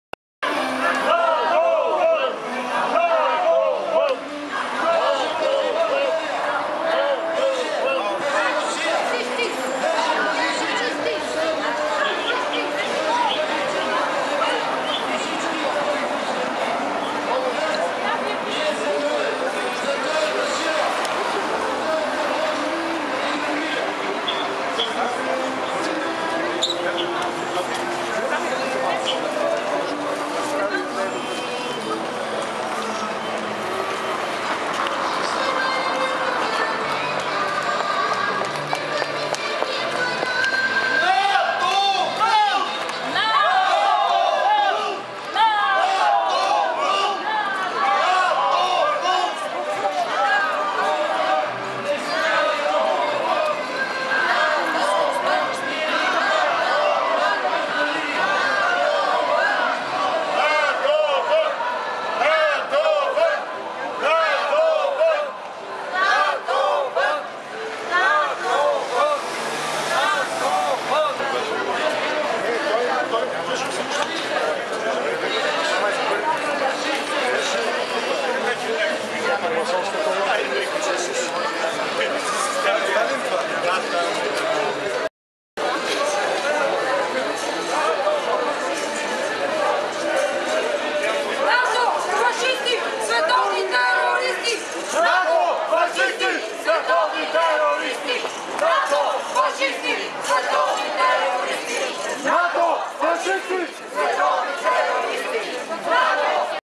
Шествие мимо русской церкви, 12-й митинг-шествие "Болгария-зона мира!" 26.04.2015
12-й митинг-шествие"Болгария - зона МИРА!", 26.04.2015.
Шествие по традиции остановилось перед русской церковью в Софии, как перед символом России. Впереди колонны болгары держали два огромных флага – Болгарии и России.  Люди скандировали: "НАТО, вон! НАТО, вон! НАТО - фашисты, мировые террористы!"

Болгары на митинге выступили с требованиями: „СТОП агрессии против Донбасса! США, ВОН из Болгарии и Европы! Болгария – ЗОНА МИРА! Европа - ЗОНА МИРА!” Против Трансатлантического торгового и инвестиционного соглашения (TTIP)

Митинг организован общественным движением Гражданская инициатива «За суверенитет Болгарии и государств во всём мире!»
ФЕЙСБУК СТРАНИЦЫ митингов-шествий „Болгария – ЗОНА МИРА”